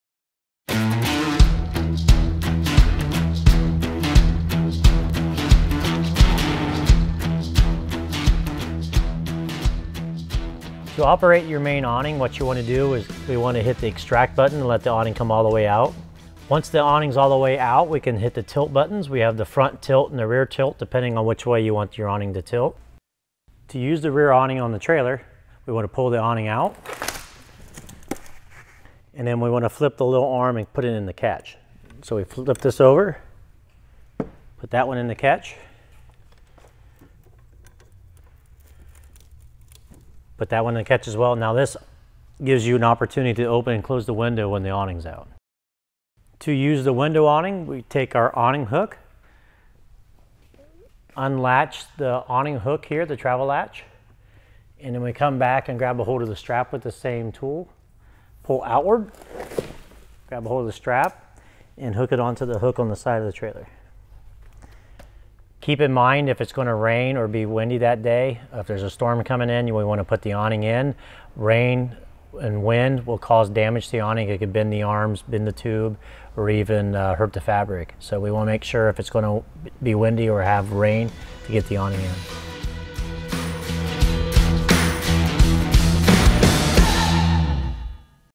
How to Use the Awning on an Airstream (Manual and Powered)
Learn how to open your Airstream’s awnings manually or from the control panel, plus find out how to prevent damage to the awnings.